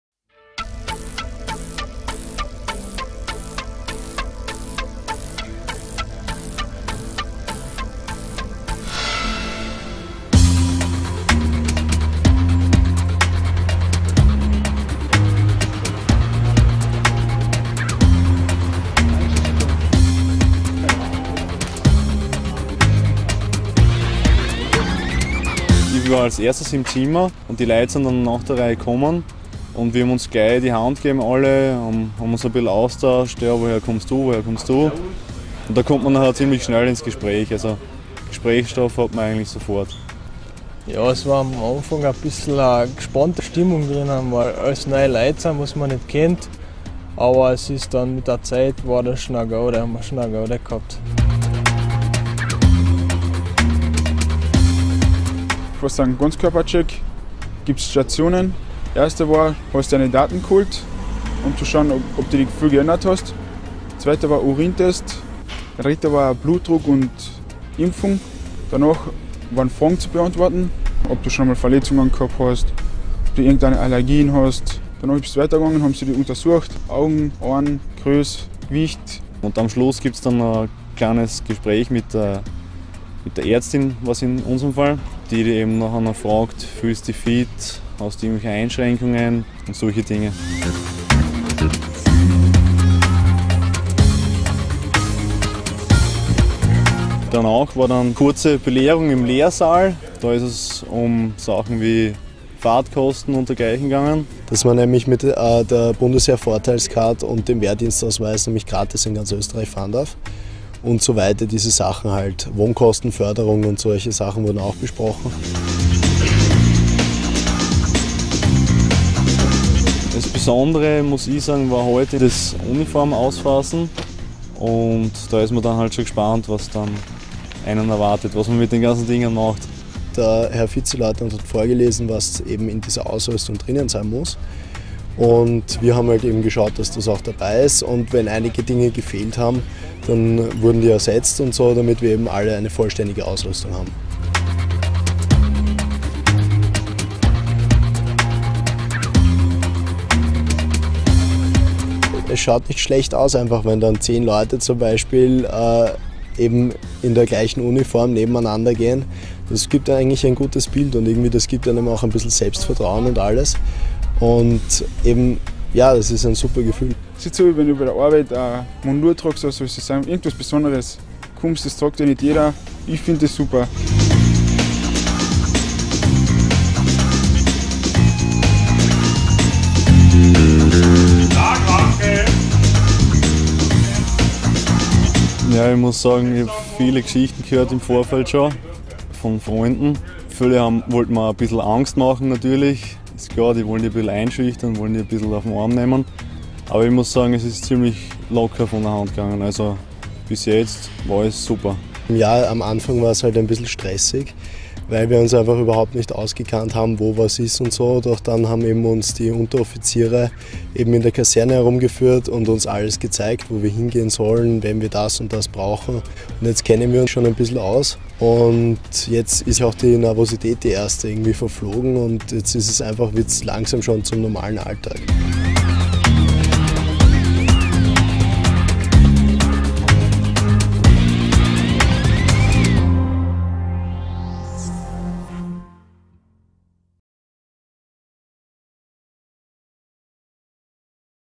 Der erste Tag beim Grundwehrdienst
Vier Grundwehrdiener erzählen über die ersten 24 Stunden beim Bundesheer.